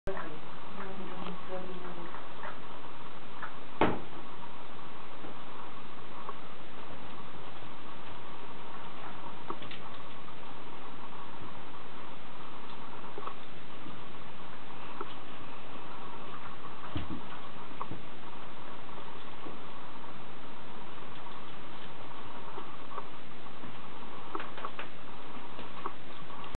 Bichon Frise Puppy: Bella licking the shower
Bella (5 months) licking the water in the shower. She loves to do this after I have a shower haha. =]